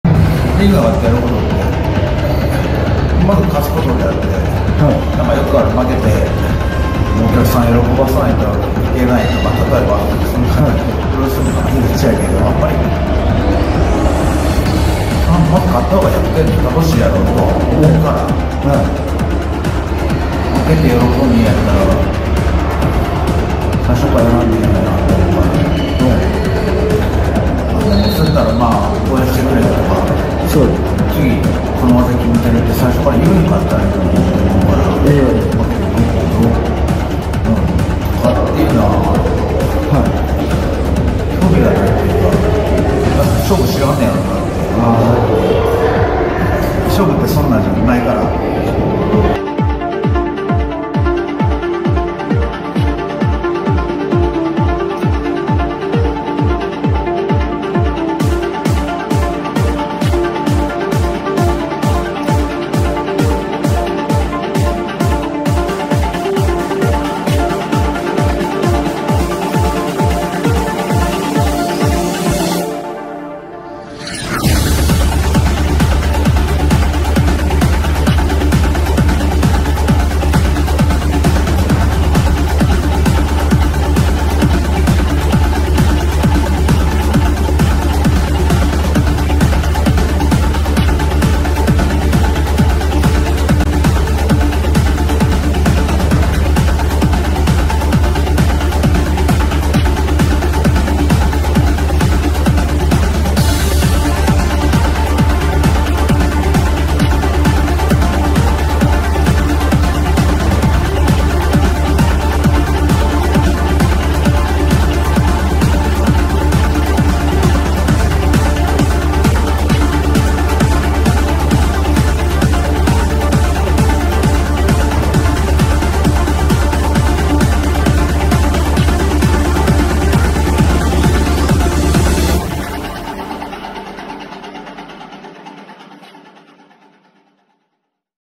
7.7夢闘派蕨大会 PV祭り 佐藤泰対伊藤崇文 PV
この闘いは“勝負は勝たなければ意味がない”という持論を持つ両者による“勝負論の闘い”なのです。

7.7夢闘派プロレス蕨大会
2024年7月7日(日) 11時30分試合開始
蕨:レッスル武闘館
全席自由:5,000円
※高校生以下、体の不自由な方は入場無料